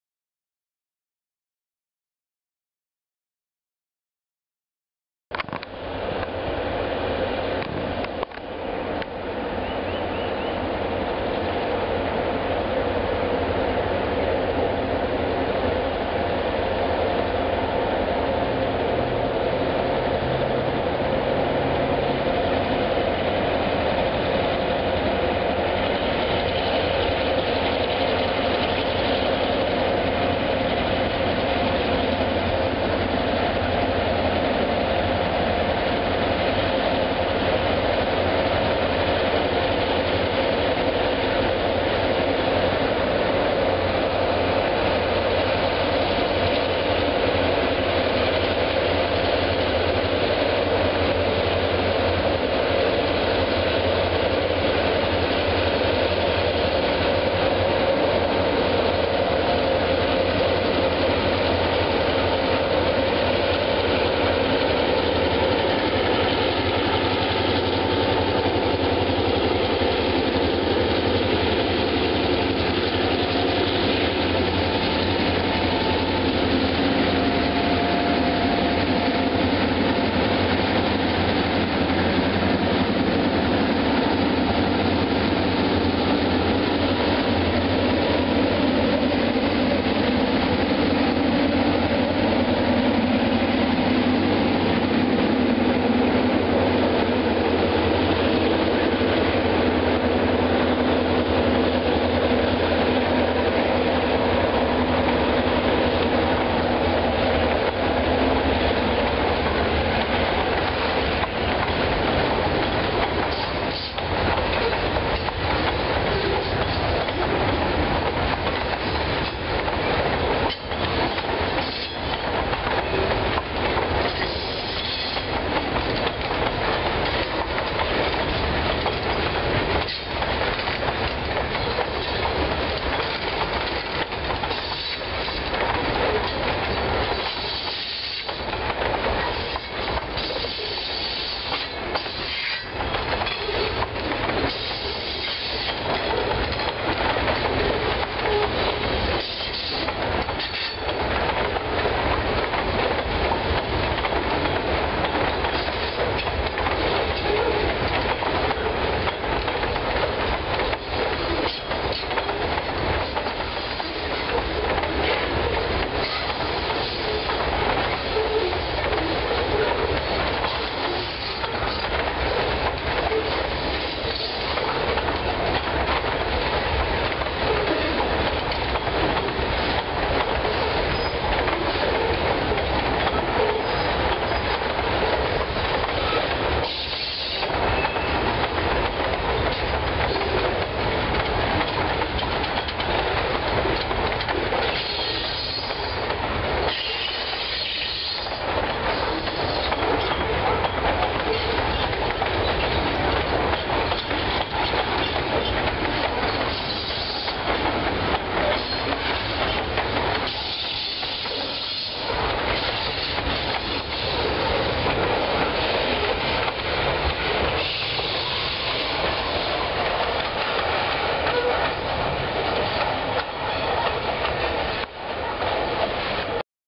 Georgia Northeastern northbound grain train in Holly Springs,GA
A 29 car long northbound Georgia Northeastern grain train works hard up the grade in Holly Springs,GA on 3/14/09.